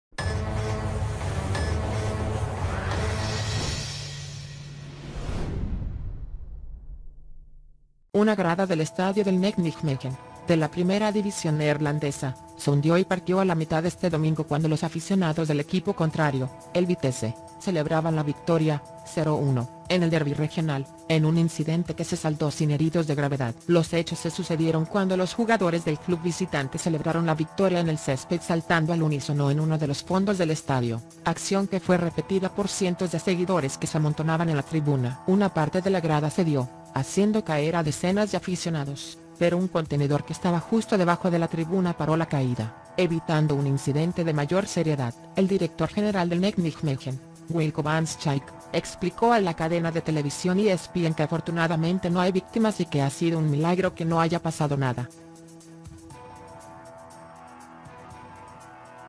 Grada del estadio del NEC Nijmegen se parte a la mitad cuando aficionados festejaban
Una grada del estadio del NEC Nijmegen, de la primera división neerlandesa (Países Bajos), se hundió y partió a la mitad este domingo cuando los aficionados del equipo contrario, el Vitesse, celebraban la victoria (0-1) en el derbi regional, en un incidente que se saldó sin heridos de gravedad.

Los hechos se sucedieron cuando los jugadores del club visitante celebraron la victoria en el césped saltando al unísono en uno de los fondos del estadio, acción que fue repetida por cientos de seguidores que se amontonaban en la tribuna.

Una parte de la grada cedió, haciendo caer a decenas de aficionados, pero un contenedor que estaba justo debajo de la tribuna paró la caída, evitando un incidente de mayor seriedad.

El director general del NEC Nijmegen, Wilco van Schaik, explicó a la cadena de televisión ESPN que “afortunadamente no hay víctimas” y que “ha sido un milagro que no haya pasado nada”.